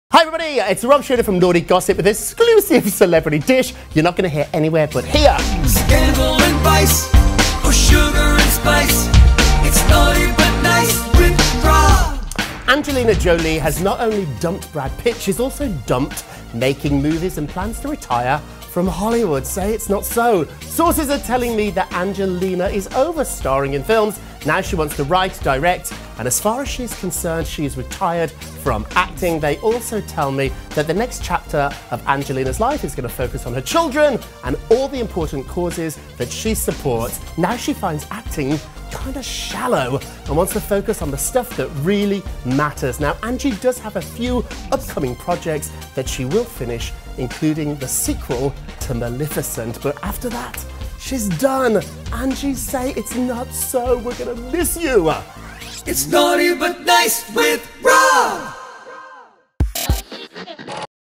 Angelina Jolie Is Retiring! | Naughty But Nice
Angelina Jolie is retiring from acting! One of her last films will be the Maleficient sequel. Find out why she's retiring above.